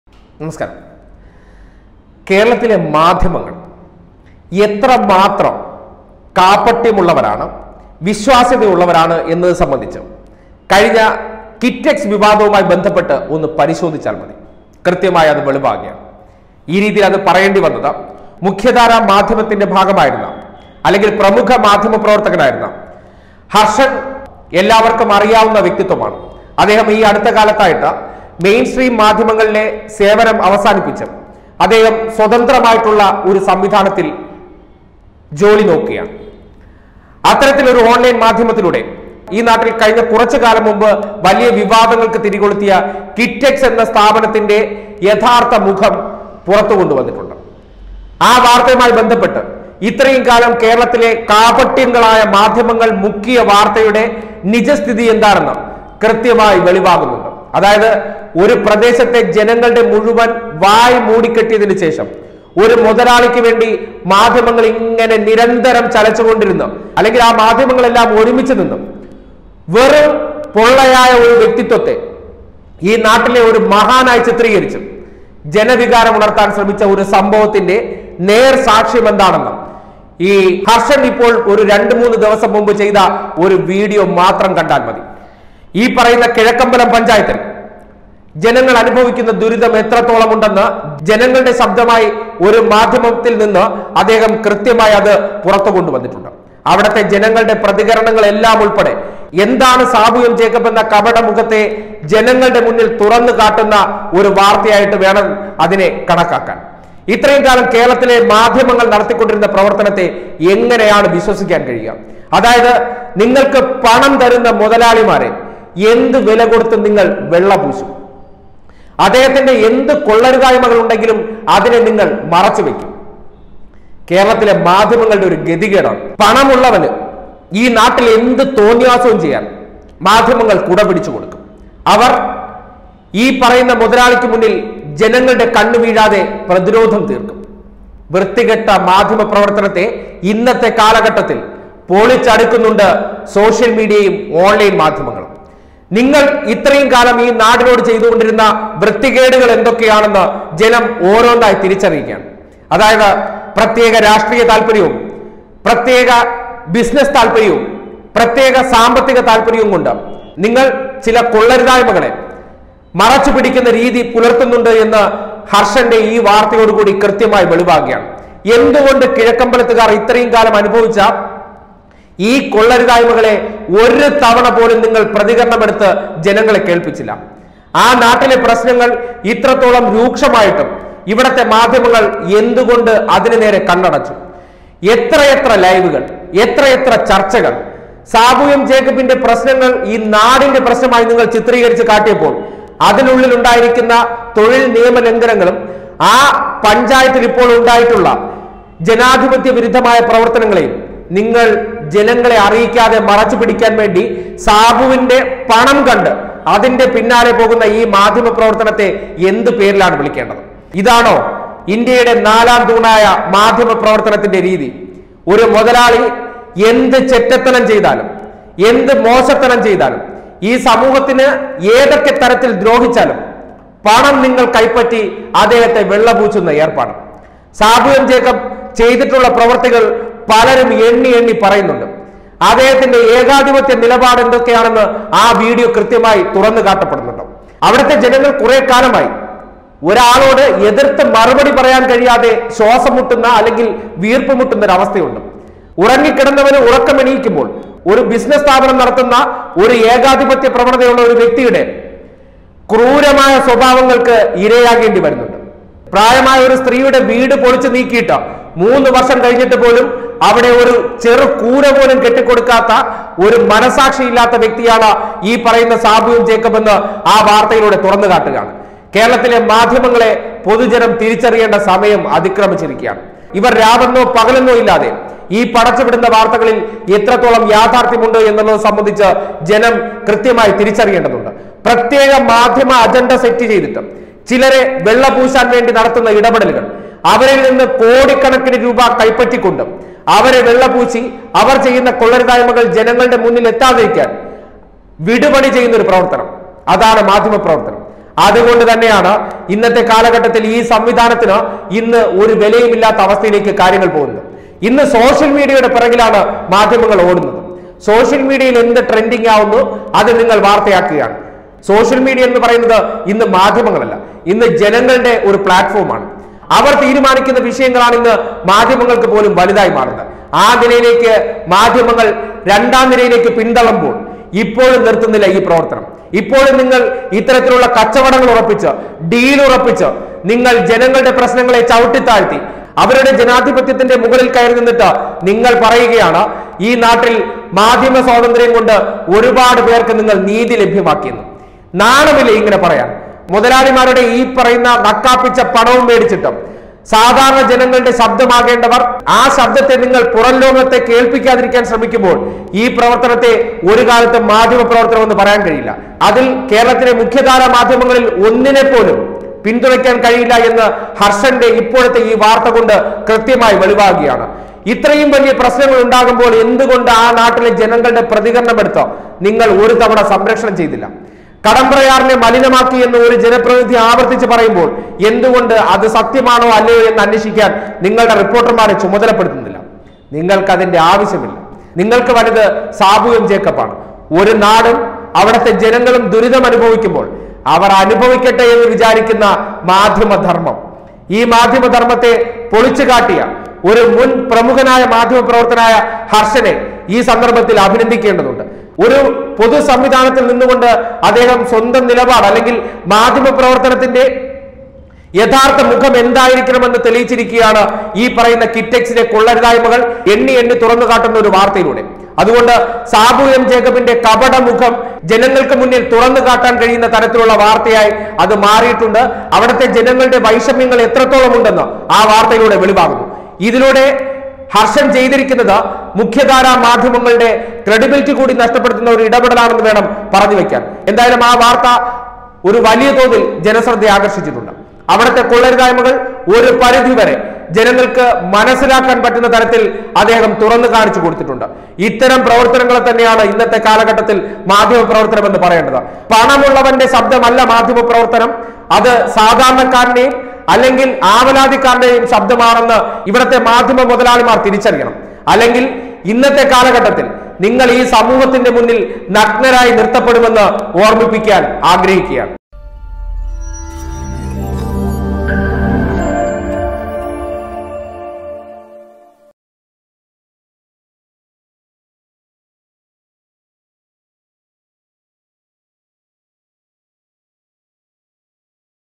സാബു മൊയലാളിയുടെ നെറികേടുകൾ തുറന്നു കാട്ടി മാധ്യമ പ്രവർത്തകൻ ഹർഷൻ; | News Updates
സാബു മൊയലാളിയുടെ നെറികേടുകൾ തുറന്നു കാട്ടി  മാധ്യമ പ്രവർത്തകൻ ഹർഷൻ; ഒറ്റുകാരായി മുഖ്യധാര മാധ്യമങ്ങൾ

Tag-Generator-Youtube-malayalam news-global